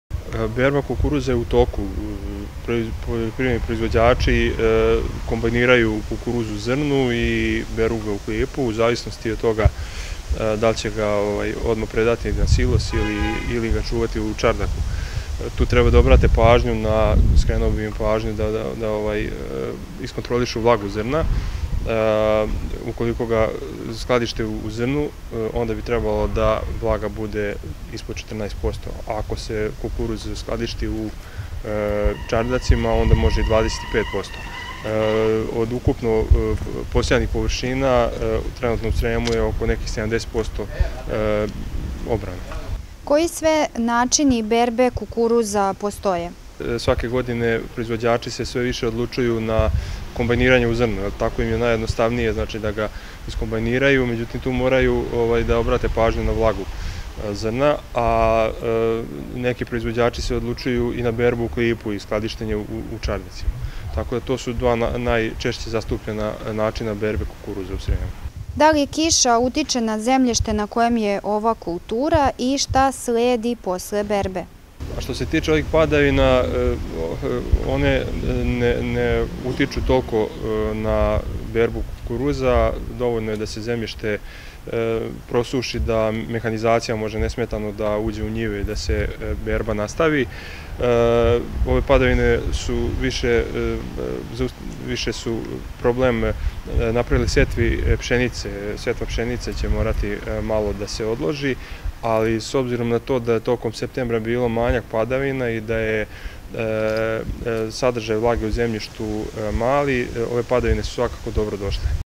Berba kukuruza u toku
Berba kukuruza prekinuta je usled nepovoljnih vremenskih prilika I obilnih kiša koje padaju ovih dana. Na više načina odvija se berba ove ratarske kulture a kakve posledice imaju padavine na ovu poljoprivrednu kulturu I na berbu, pitali smo stručnjaka doktora Vladimira Marića iz Poljoprivredne stručne službe u Sremskoj Mitrovici.